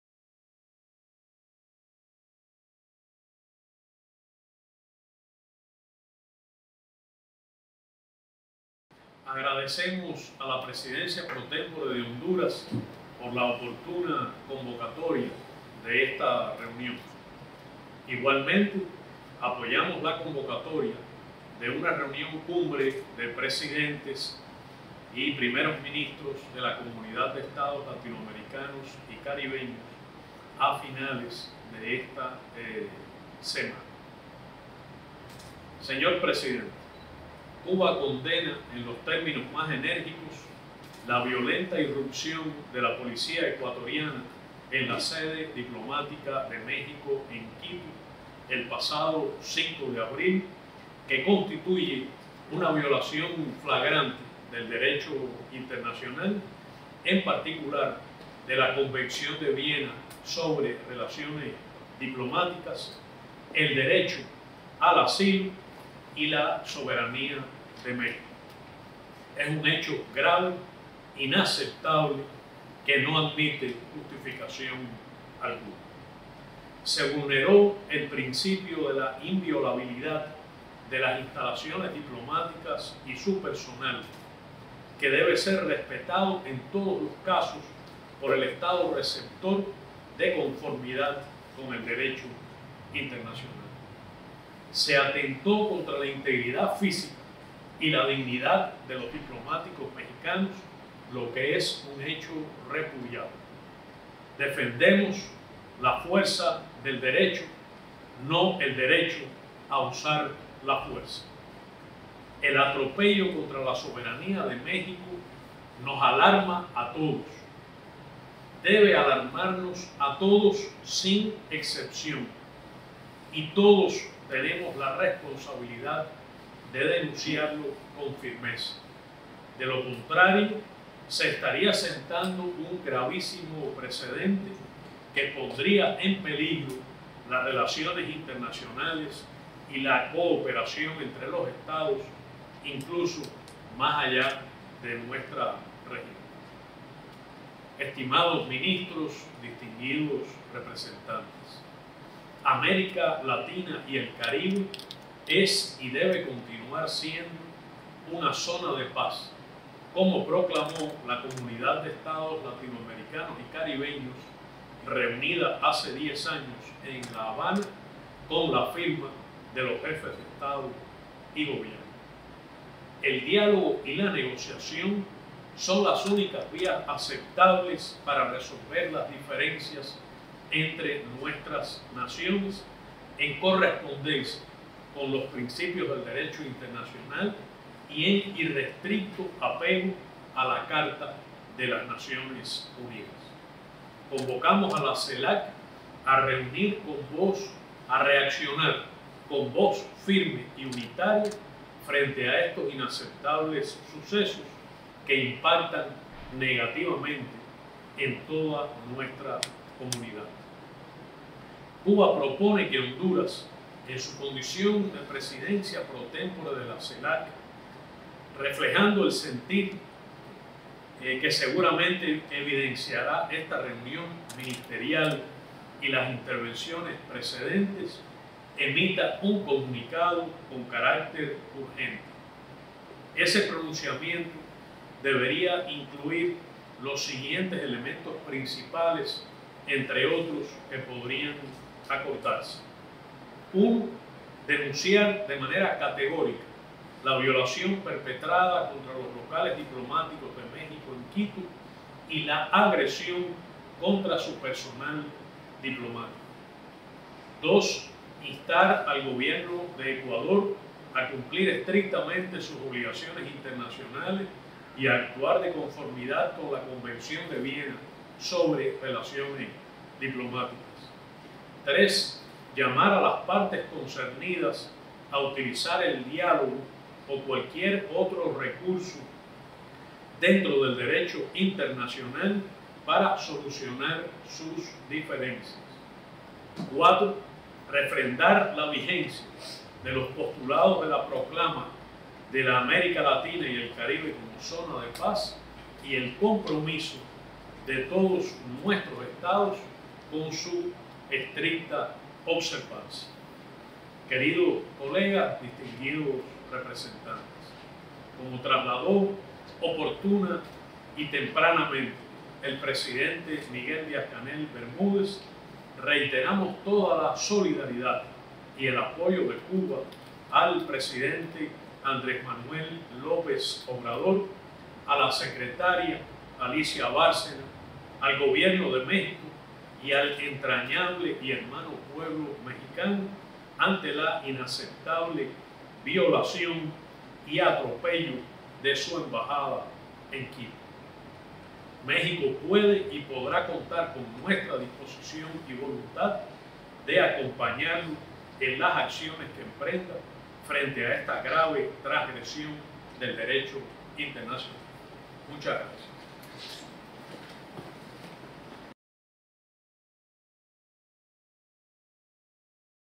Bruno Rodríguez Parrilla: Defendemos la fuerza del Derecho, no el derecho a la fuerza
Bruno Rodríguez Parrilla, Ministro de Relaciones Exteriores: “Defendemos la fuerza del Derecho, no el derecho a la fuerza”,  vea su intervención en la reunión extraordinaria ministerial de la Celac para examinar los recientes hechos en la Embajada de México en Ecuador.